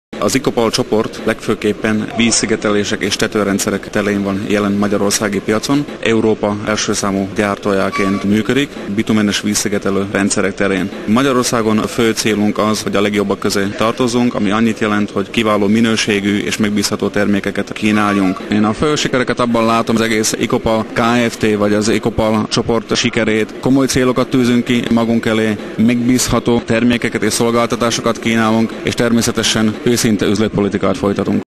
Icopal Kft. - Snírer Krisztián Ügyvezető Igazgató
- Nyilatkozik a(z) Icopal Kft. CÉG CÉGVEZETŐJE Snírer Krisztián Ügyvezető Igazgató! Icopal Kft. A LEGJELENTŐSEBB MAGYAR CÉGEK KÖZÖTT!